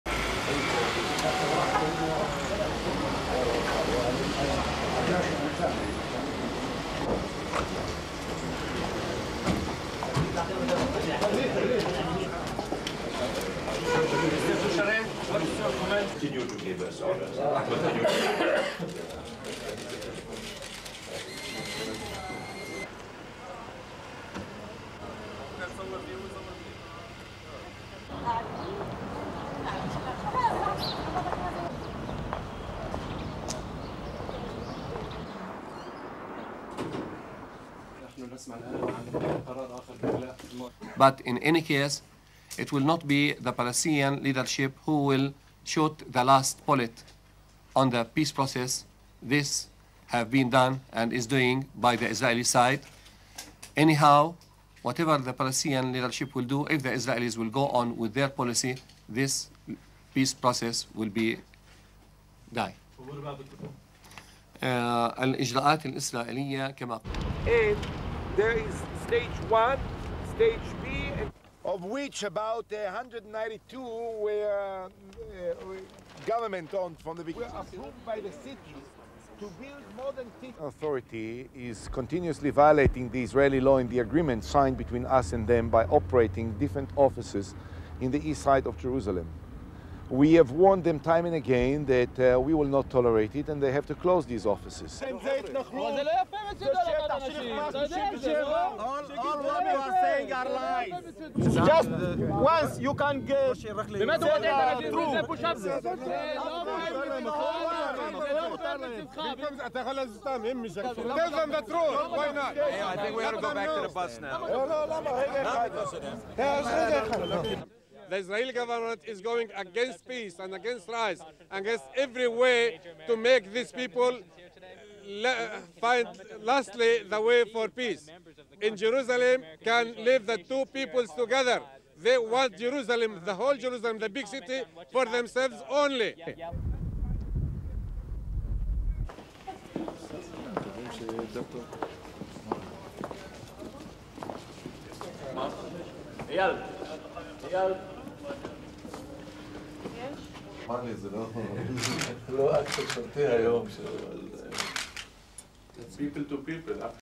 MIDDLE EAST: REACTION TO CLOSURE OF 4 PALESTINIAN OFFICES UPDATE
A senior PLO official, Faisal Husseini, warned on Wednesday that the closure of 4  Palestinian offices in east Jerusalem could plunge the city into violence.

The offices - which were run by the Palestinian Authority - are expected to be shut  down by the Israeli government within the next 24 hours.

Husseini later met with the French Foreign Minister, Herve de Charette, who is currently  in Israel.

The top P-L-O official in Jerusalem met with the French foreign minister, Herve de  Charette, Wednesday.

But the meeting was held not in Orient House -- the Palestinian's unofficial foreign  ministry in the city -- but in a nearby hotel.

Israel refuses to allow foreign official visits to the building.

Israeli police are expected to order the P-L-O to shut down 4 Palestinian offices during  the course of Wednesday.

But it's not yet clear which of the Palestinian sites in east Jerusalem will be subject to  the closure orders.

It's uncertain whether they include Orient House.

Such a move could inflame relations with the Palestinians, already tense after Israel's  decision last week to build a large Jewish neighbourhood in east Jerusalem.

Husseini warned at a press conference earlier in the day that the closures were putting  the peace process in jeopardy.

SOUNDBITE: (English)
"But in any case it will not be Palestinian leadership who will shoot the last bullet on  the peace process, this has been done and is doing by the Israeli side. Anyhow  whatever the Palestinian leadership will do if the Israeli's will go on with their policy, this  peace process will be gone."
SUPER CAPTION: Faisal Husseini, top PLO official in Jerusalem

Under the Israel-P-L-O agreements, the Palestinian Authority is not permitted to  operate in Jerusalem.

But Israel has repeatedly accused the authority of opening agencies in Jerusalem under  various guises, and Israel has forced some of them to shut down.

The possible closure of the Palestinian offices comes as tensions over East Jerusalem  have already been put under considerable strain.

The Israeli government insists it will go ahead with plans to build a Jewish settlement of  6-thousand-500 houses on a site called Har Homa - known as Jabal Abu Ghneim in  Arabic.

On Wednesday, Jerusalem's mayor, Ehud Olmert, toured the Har Homa site with  members of the American conference of presidents.

SOUNDBITE: (English)
(The Palestinian) authority is continuously violating the Israeli law in the agreement  signed between us and them by operating different offices in the East side of  Jerusalem. We have warned them time and again that we will not tolerate this and they  have to close these offices.
SUPER CAPTION: Ehud Olmert, Jerusalem mayor

However, the level of emotion that the site has sparked was evident as
a group of Israeli Arab legislators confronted Olmert.

Members of the American conference of presidents looked decidedly uncomfortable as  the crowd closed in on them.

SOUNDBITE:
The Israeli government is going against peace and against lies and against every way to  make these people find lastly the way to peace. In Jerusalem can live the two people  together. They want Jerusalem, the whole Jerusalem, the big city for themselves only.
SUPER CAPTION: Abd el-Malek Dahamshe, Israeli Arab legislator

Israel's decision appeared to be timed to coincide with the weekend's planned Israeli  West Bank troop pullback to minimise the chances of a severe Palestinian reaction.